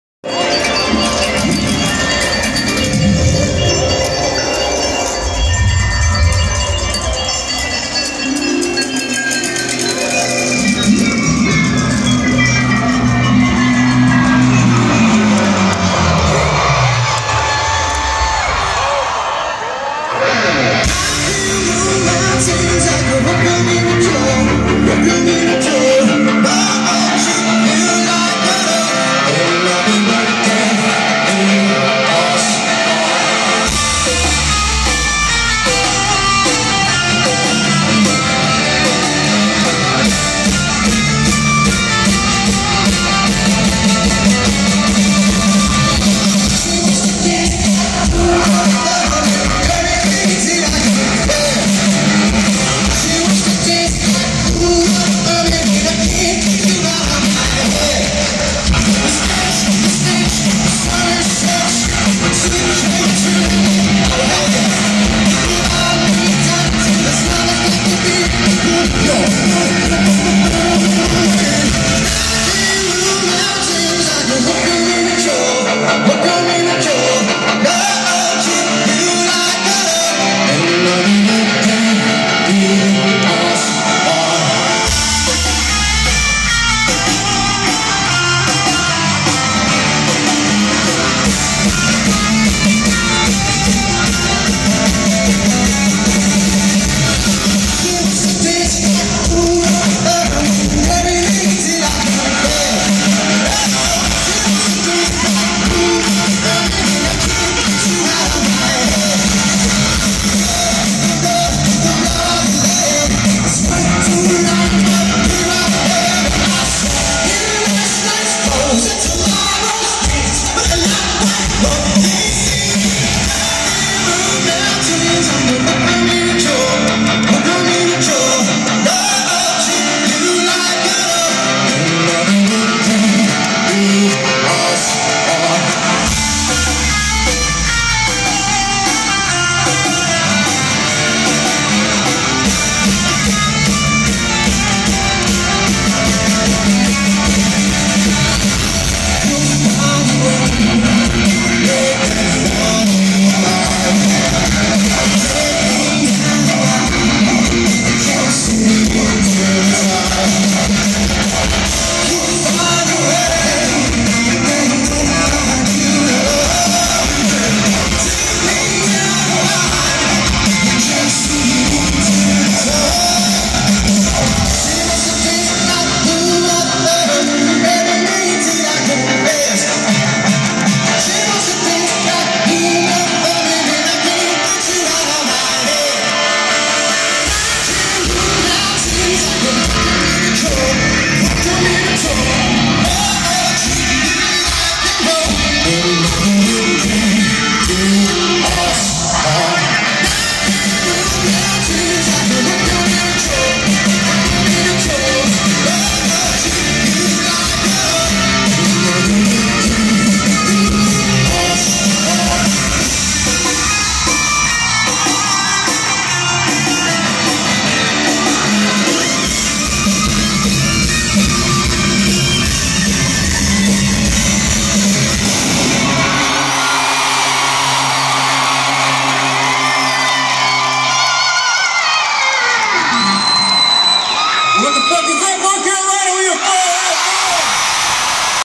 Fall Out Boy Live From PNC Arena Charlotte NC view from Lawn 7/21/23
view from lawn